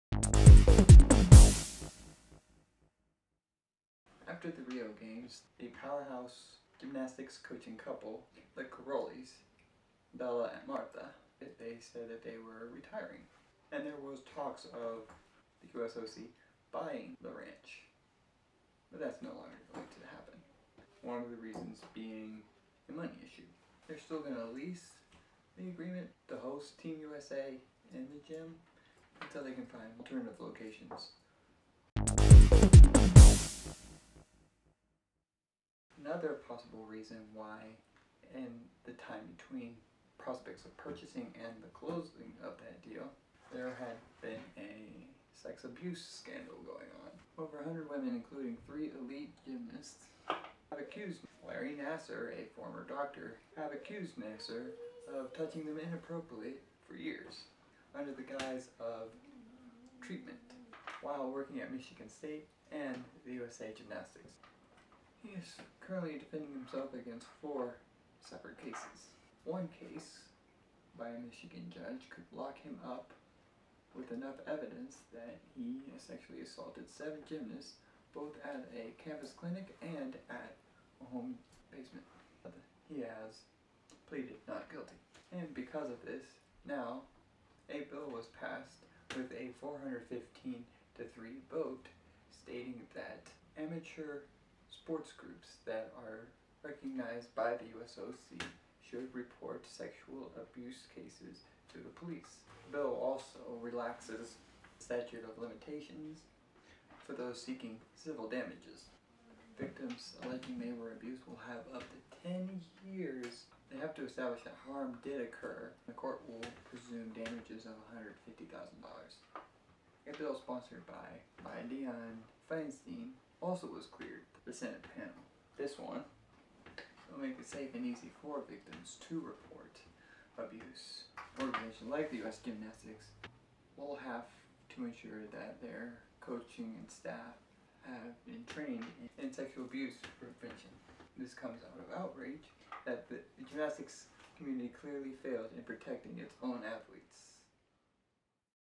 Not cool USA Ggynastics
the USA Gymnastics organization ditches the Karolyi ranch purchase for 2 of many reasons. the last one is infuriating.